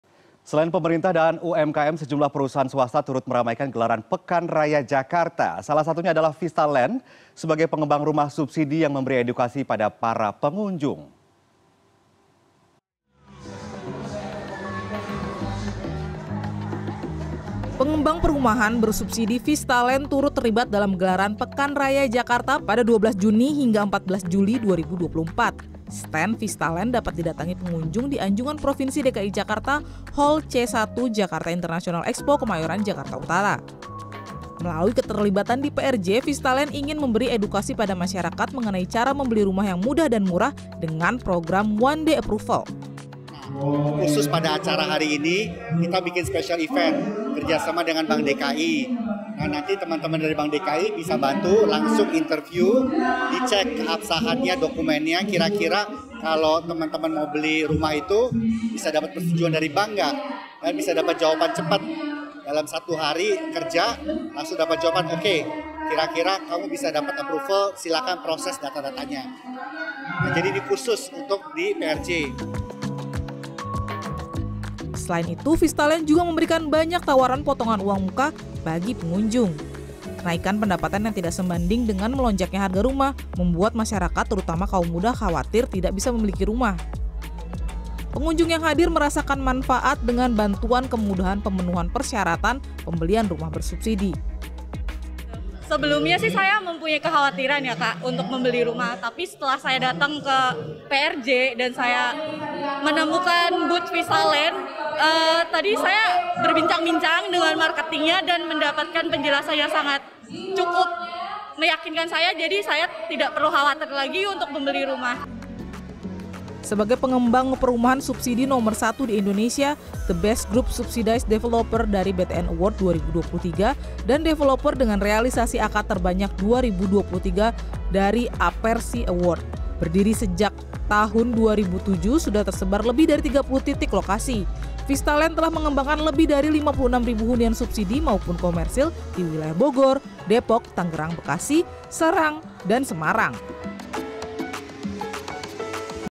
Vista Land di Pekan Raya Jakarta
Selain pemerintah dan UMKM, sejumlah perusahaan swasta turut meramaikan gelaran Pekan Raya Jakarta. Salah satunya adalah Vista Land, sebagai pengembang rumah subsidi, yang memberi edukasi pada para pengunjung.